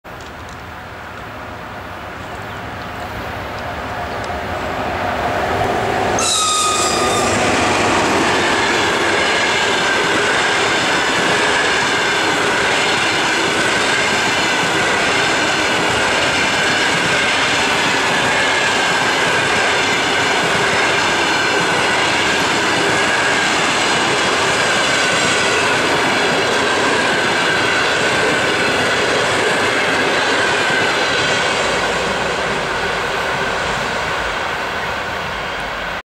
Электровоз ЭП1М-527 с поездом №140 Новосибирск — Адлер
Электровоз ЭП1М-527 (приписан ТЧЭ-3 Россошь Ю-ВОСТ) со скорым поездом №140 Новосибирск — Адлер, ст. Локомотивстрой, г. Новочеркасск, линия Лихая - Ростов-на-Дону, Ростовская область, Северо-Кавказская железная дорога, Россия, 13:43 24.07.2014 г.

Совместные съемки с Алексеем Царевым (em2i016).